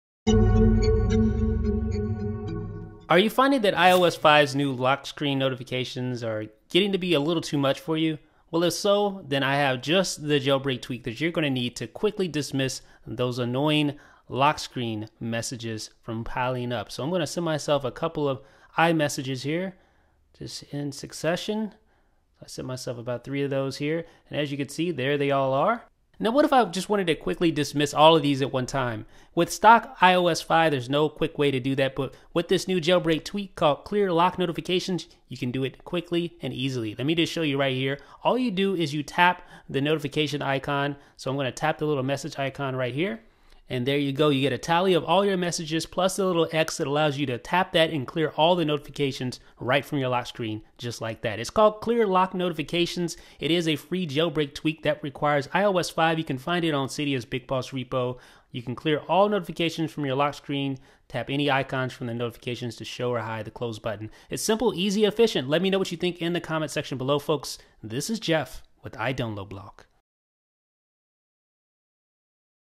ClearLockNotifications
Name: ClearLockNotifications
Description: Clear the iOS 5 lock screen notifications with just a couple of taps
Price: Free
Repo: BigBoss
Developer: Moeseth

About iDB